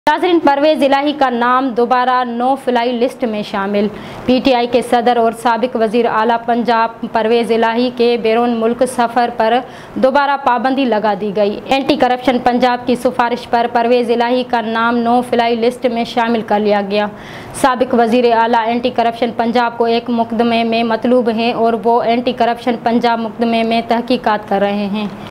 NEWS CH PERWAIZ ILLAHI 28-10-24
پرویز الہیٰ کا نام دوبارہ نو فلائی لسٹ میں شامل